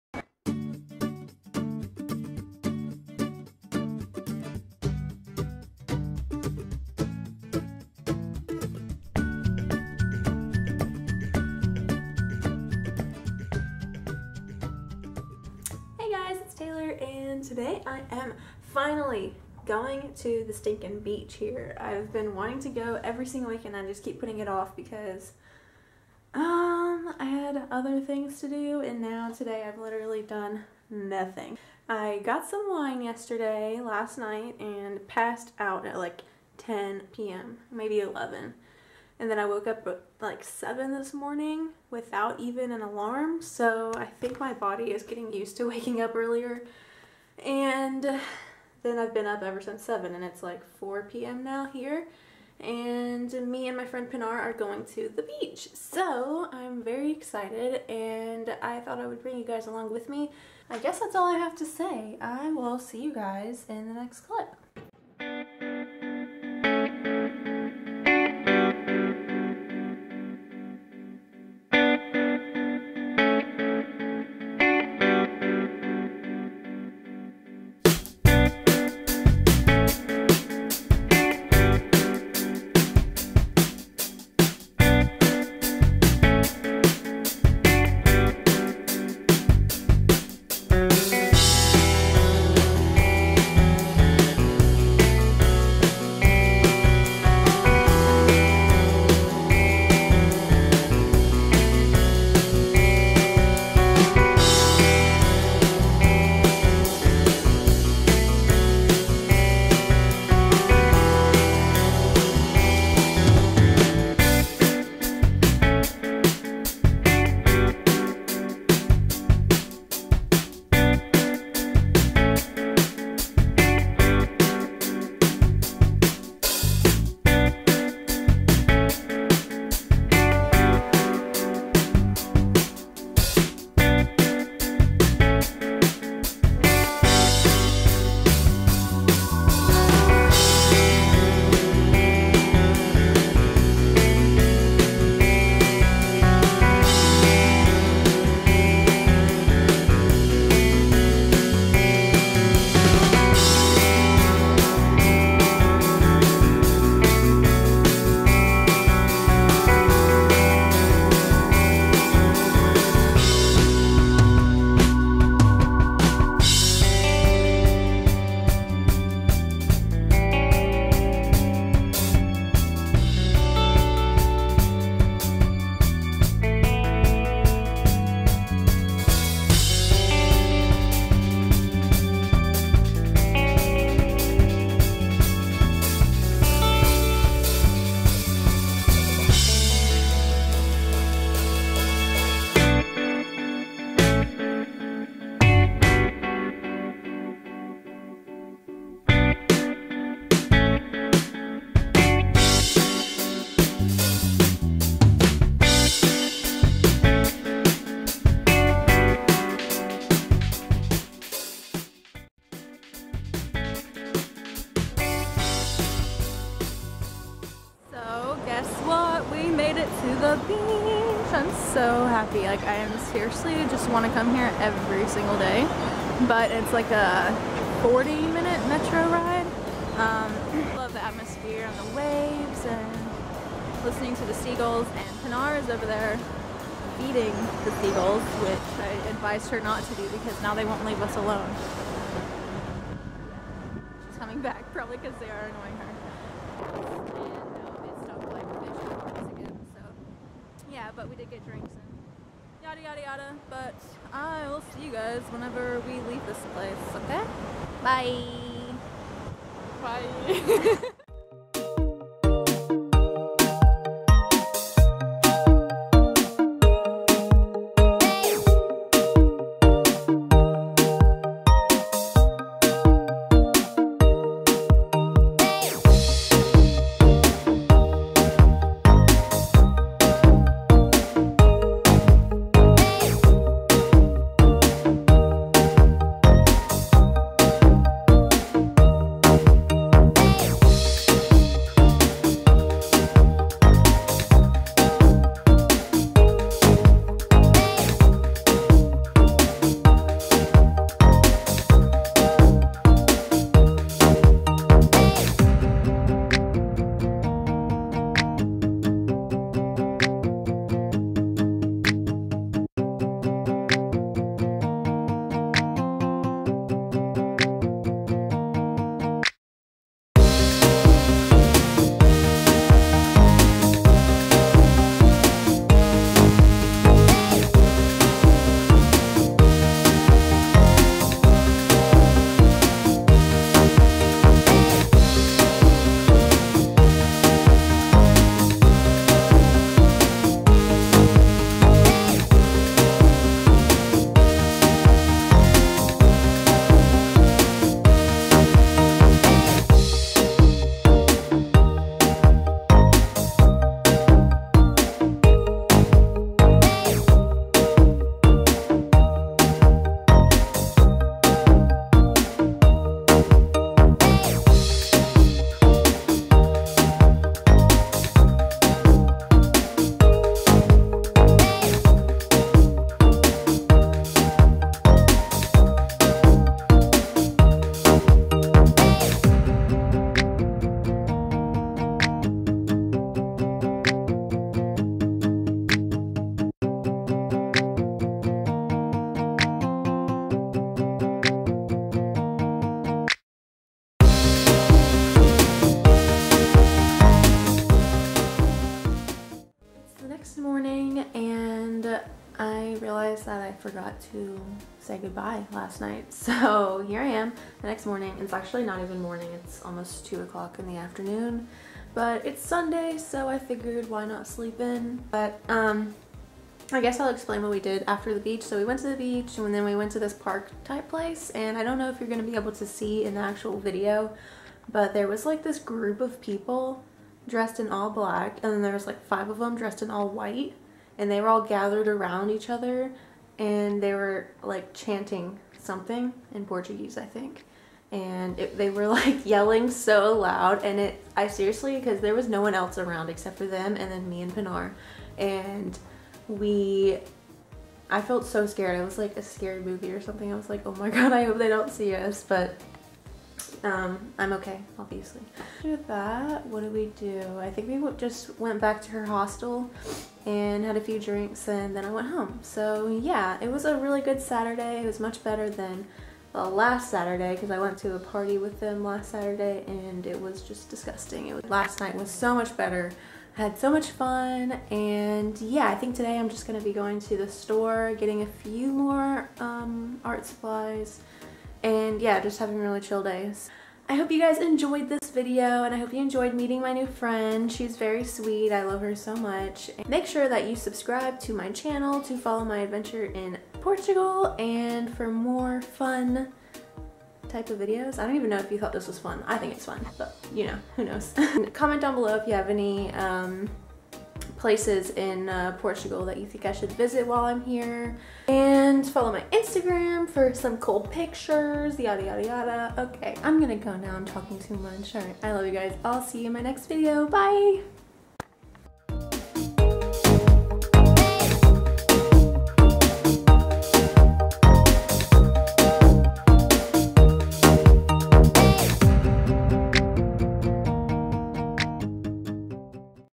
BEACH DAY | ft. Pinar!
OPEN FOR THE GOODS~~~

Hey guys! This is just a fun video introducing my best friend here, Pinar, as we go to the Matosinshos Beach here in Porto, Portugal!! It was a lovely time, and I hope you all enjoy coming with me on this adventure! ❤

❤ Come with me on my 6-month adventure to Portugal ❤